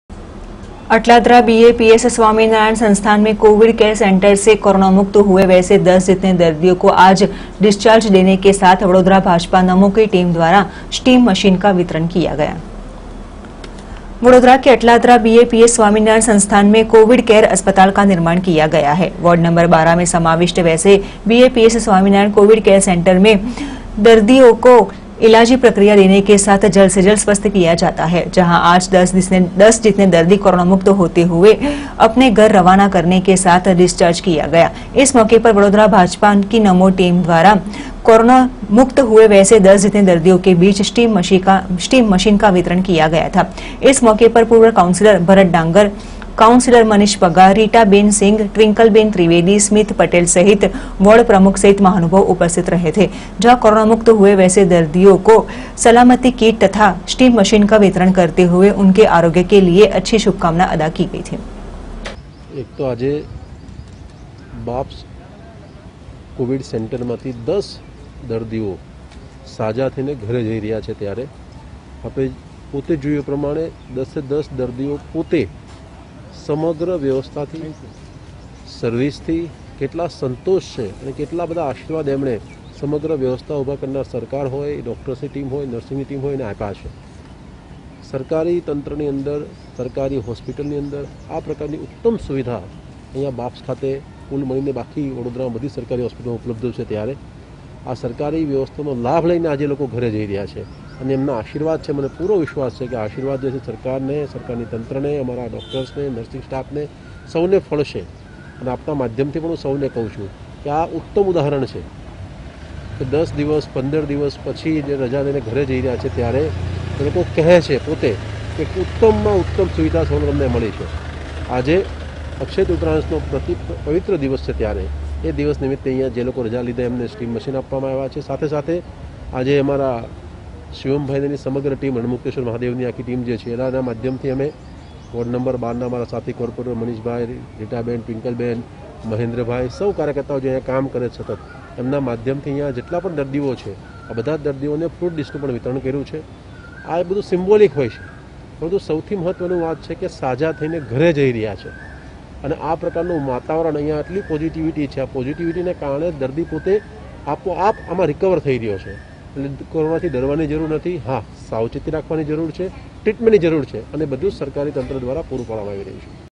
Ibn news vadodara
अटलादरा BAPS कोविड केर सेंटर से स्वस्थ हुऐ 10 दर्दि को वडोदरा भाजपा नमो टीम द्वारा स्टीम मशीन का वितरण